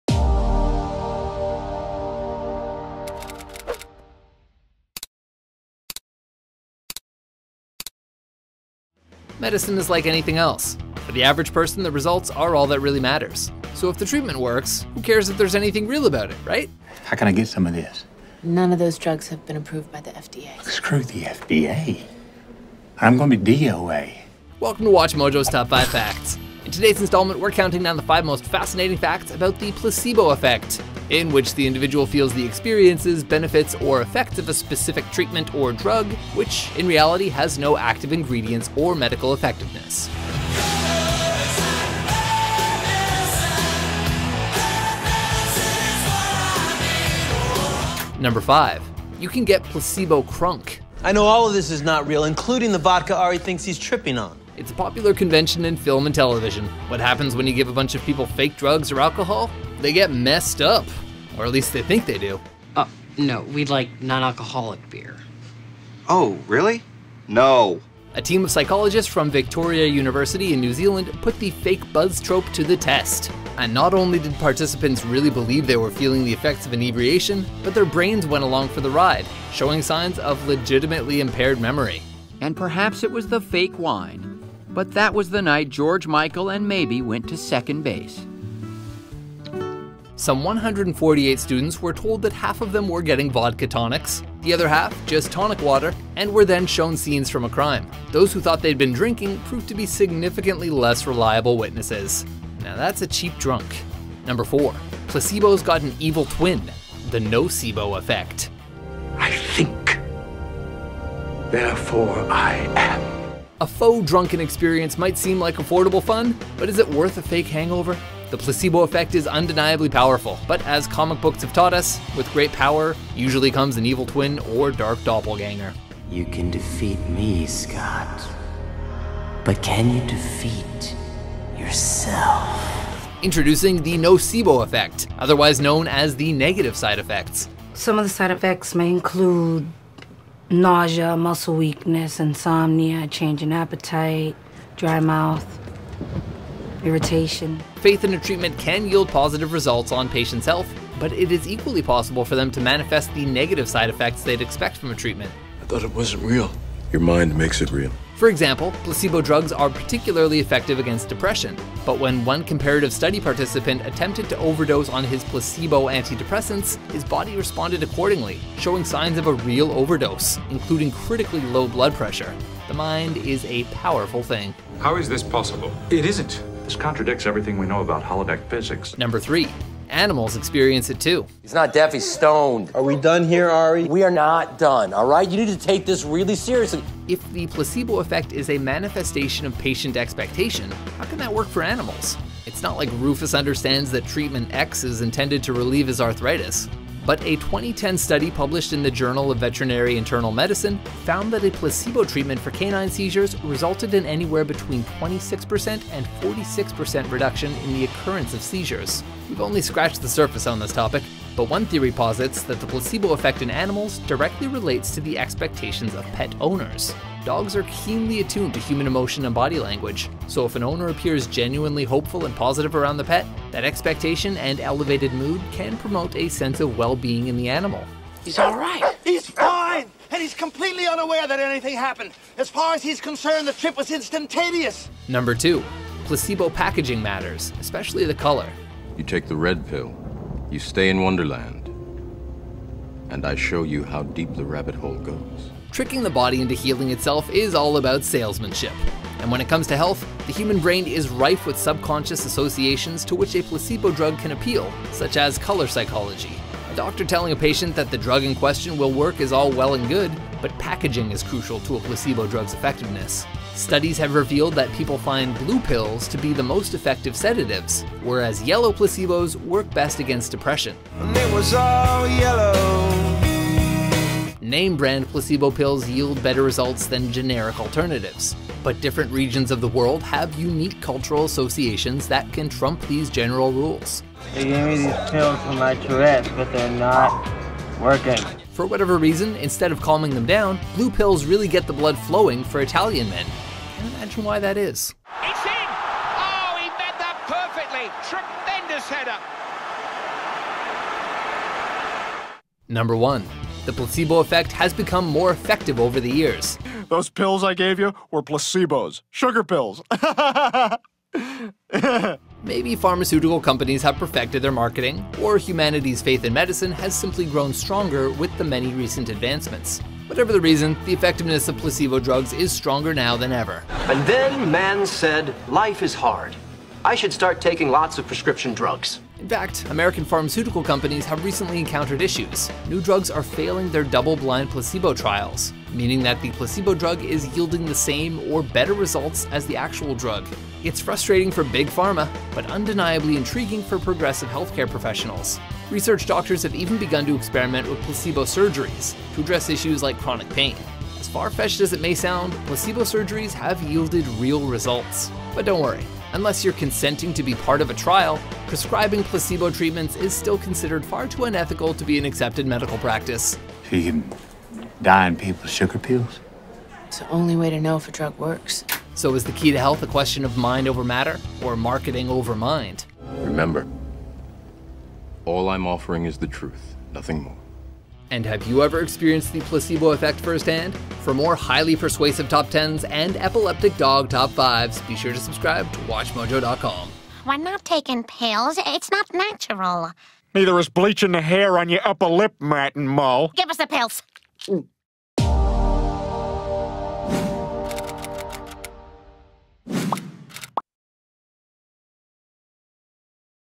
Top 5 Unbelievable Placebo Facts
If medicine works, who cares if it has any actual medicine in it, right? The science behind the placebo effect is not very well understood, but some of its implications are incredible!

Special thanks to our users MikeyP and MikeMJPMUNCH for submitting the idea on our Interactive Suggestion Tool